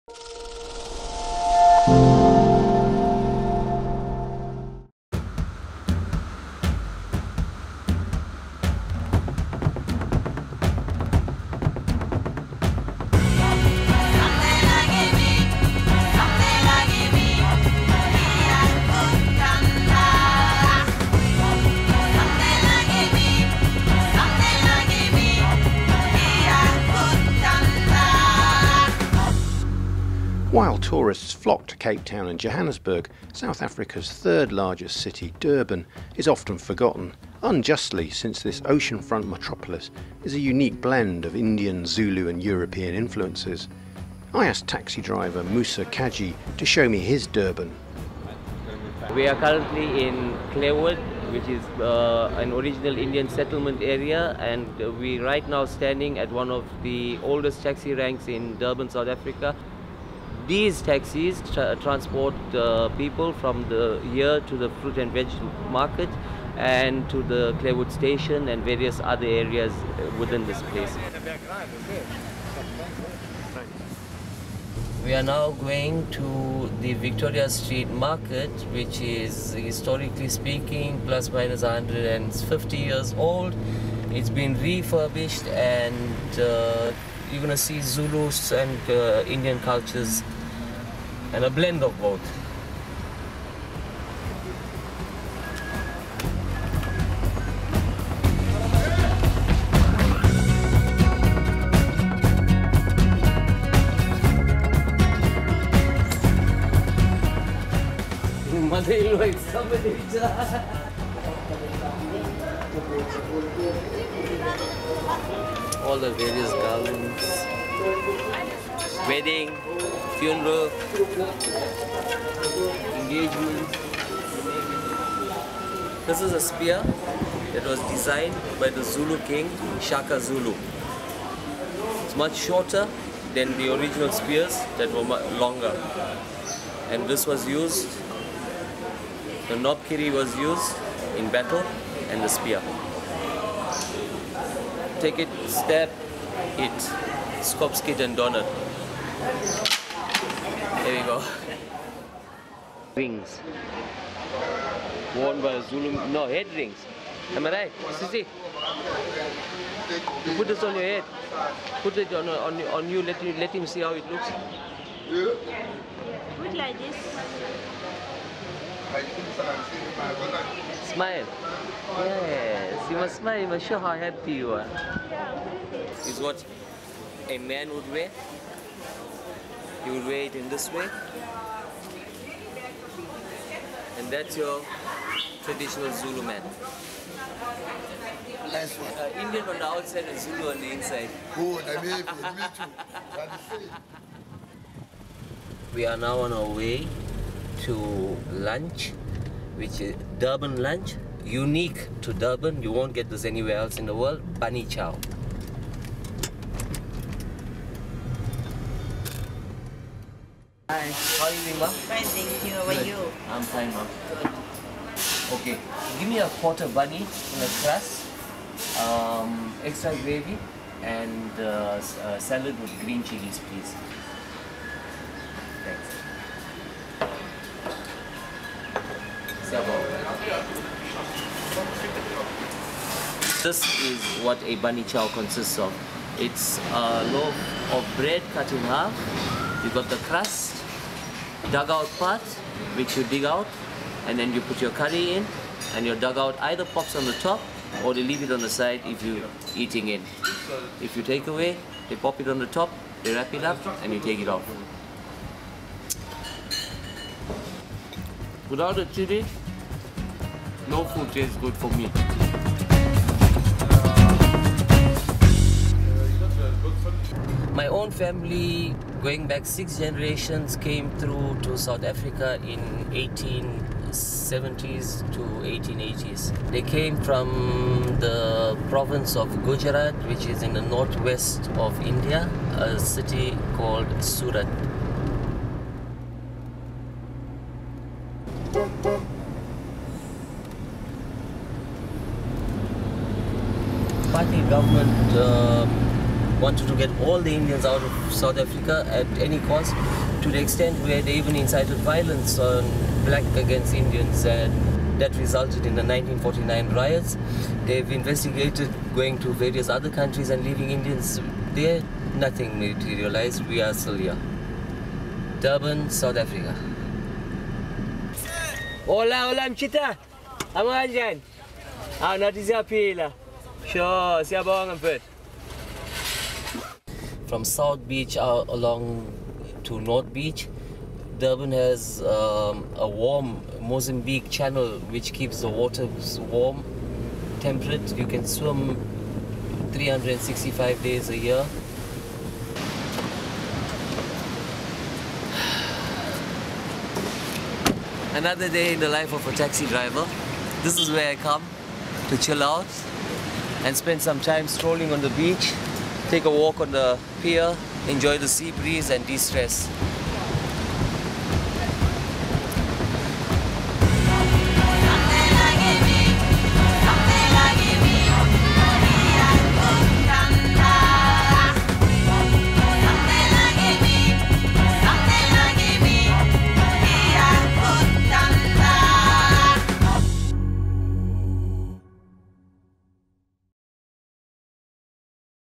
A taxi driver's tour of Durban, South Africa
South Africa's third-largest city is a unique blend of Zulu, Indian and European influences. Taxi driver Moosa Kajee takes Kevin Rushby on a tour of his city, stopping at the Victoria Street market for souvenirs, a cafe serving local fast food favourite Bunny chow, and the beachfront where he comes to chill out at the end of the day.